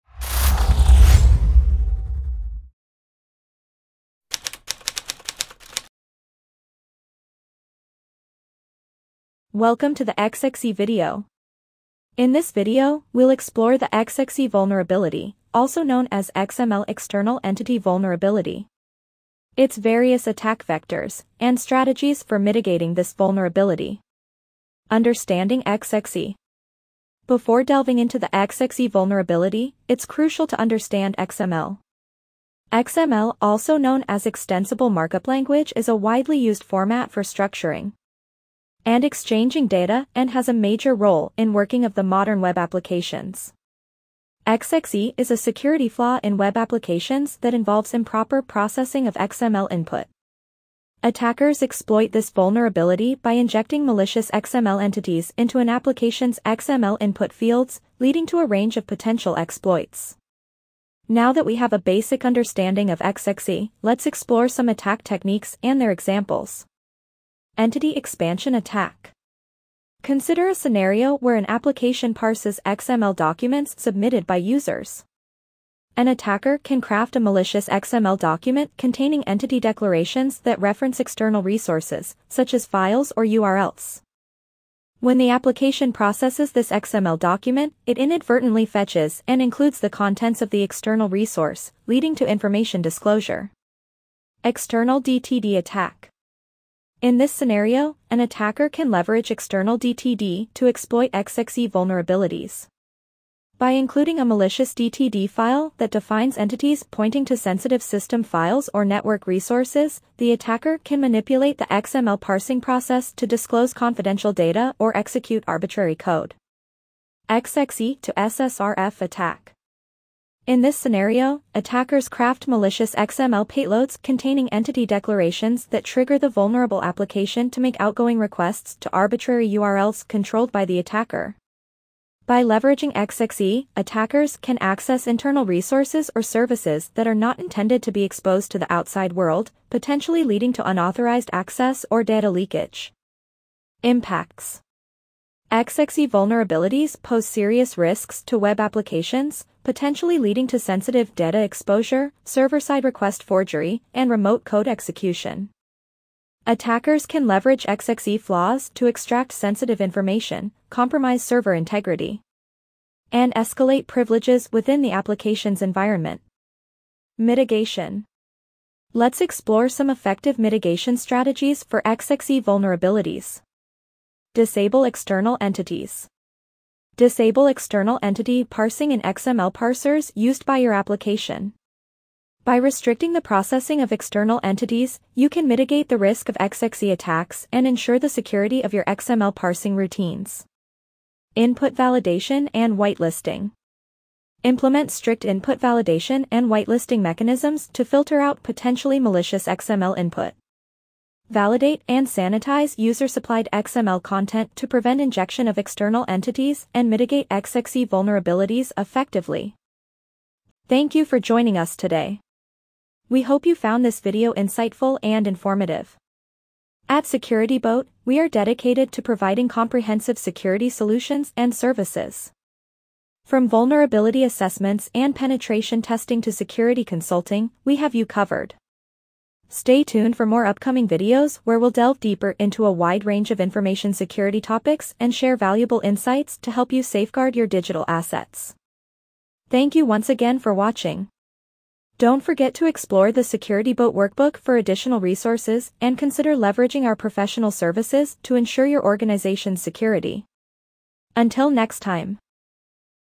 XML External Entity (XXE) Vulnerability
XXE or XML External Entity injection is a critical web security vulnerability that can lead to data exposure, server-side request forgery (SSRF), and potential compromise of the entire system. In this video, we'll dive into what XXE is, how it works, and practical examples to help you understand the risks associated with this vulnerability.